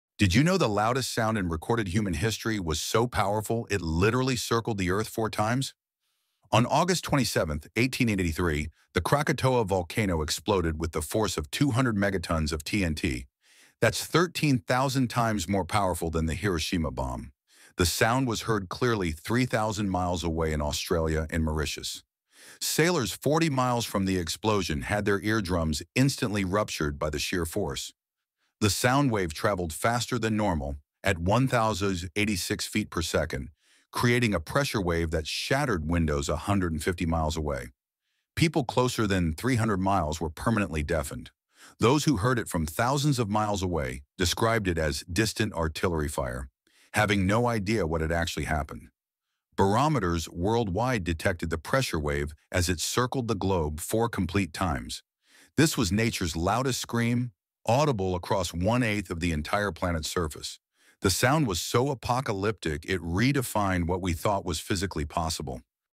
The Loudest Sound in History Circled Earth 4 Times
Krakatoa was INSANE. Heard 3000 miles away 🌋